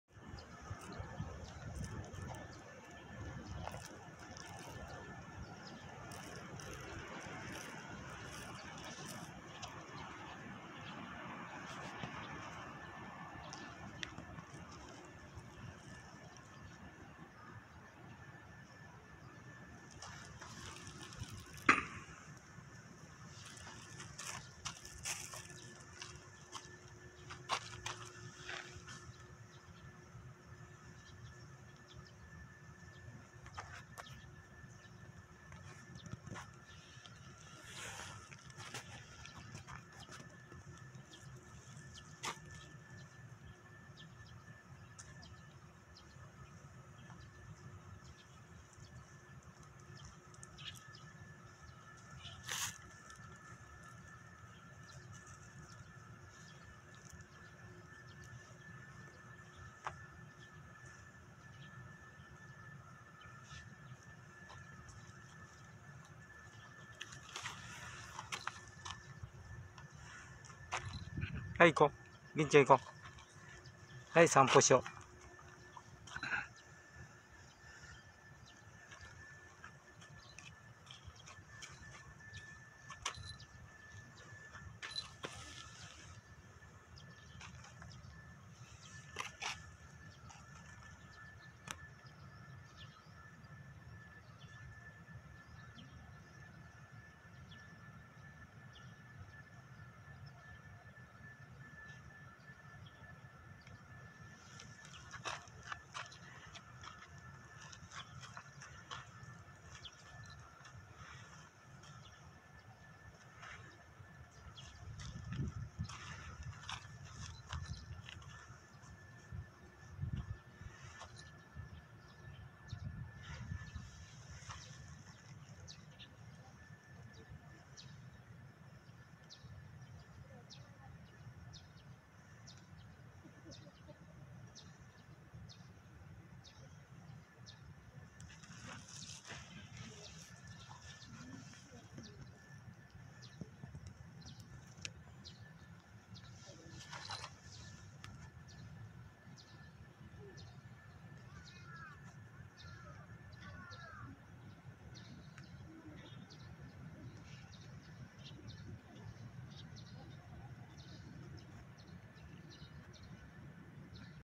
朝ごはん食べて散歩
肉の缶詰と、ササミと野菜を炊いたごはんを沢山食べて元気に今日は歩きました。